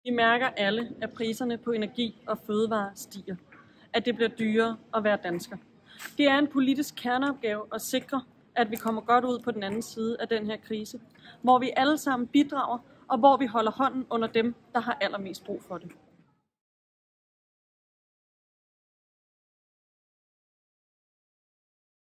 Der skal være råd til hverdagen - Fie Hækkerup (A)
DER SKAL VÆRE RÅD TIL HVERDAGEN!
Vi mærker alle, at priserne på el, varme og fødevarer stiger. At hverdagen bliver dyrere.
Rigtig mange danskere er hårdt ramt på økonomien og har svært ved at få budgettet til at hænge sammen i hverdagen. Det valg, vi står over for, bliver et tryghedsvalg. Det er en politisk kerneopgave at sikre, at vi kommer godt ud på den anden side. Hvor vi alle bidrager, og vi holder hånden under dem, der har mest brug for det.
Det handler om, at alle skal have råd til hverdagen, og det handler om, at Danmark skal trygt igennem de usikre tider.
Derfor håber jeg at I vil give mig jeres stemme, så vil jeg arbejde for at, alle får råd til hverdagen.